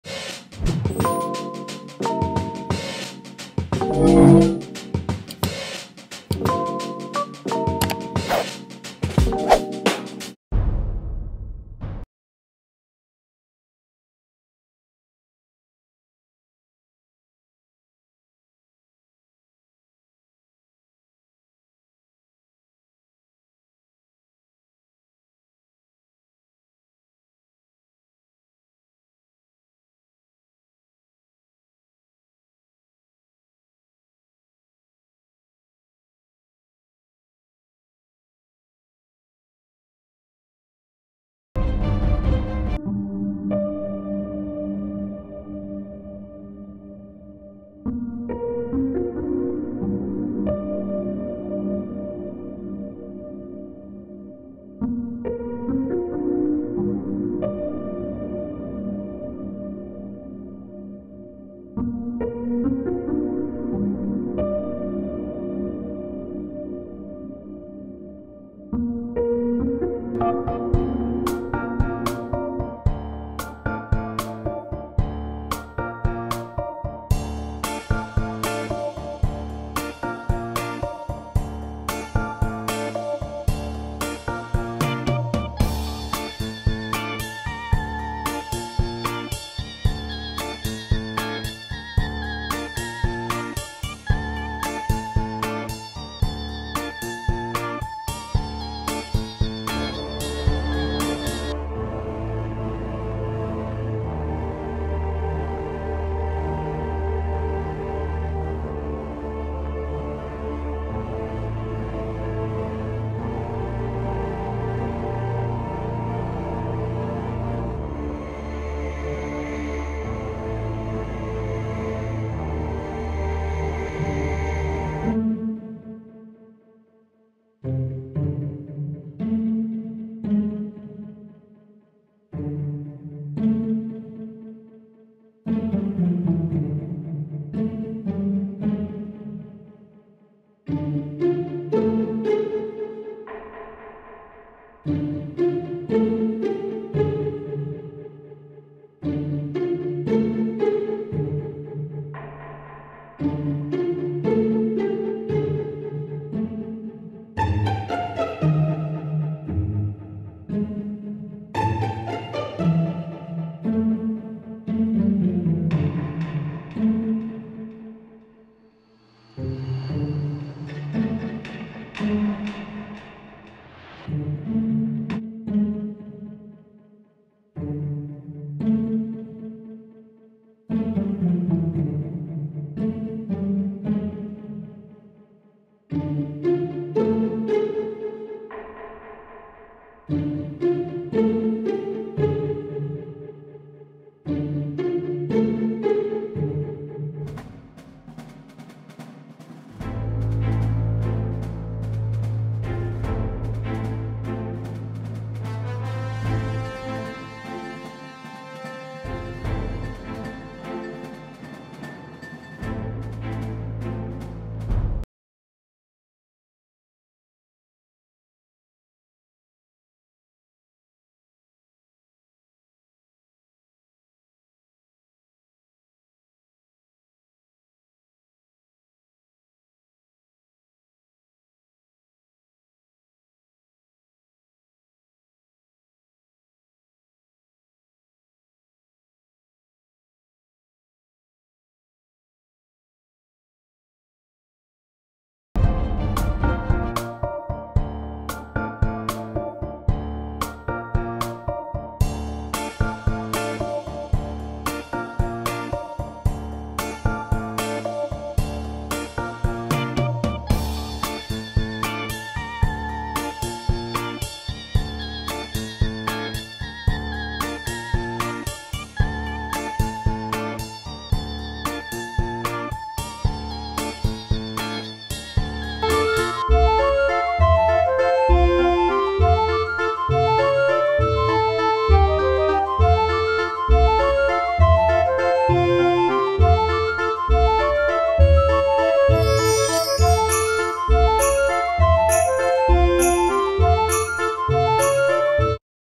Entenda a TRETA entre Charli e Dixie D'Amelio vs Addison Rae
Entenda o que aconteceu entre Charli e Dixie D'Amelio e Addison Rae, depois de uma página divulgar curtidas antigas de Addison.

Acho mto paia ficar divulgando coisa antiga pra queimar os outros.

Acabei de perceber q estou amplificando essa ação lol, mas é só para informar quem quer saber o que aconteceu entre as três. No fim das contas, parece que já está tudo bem entre elas.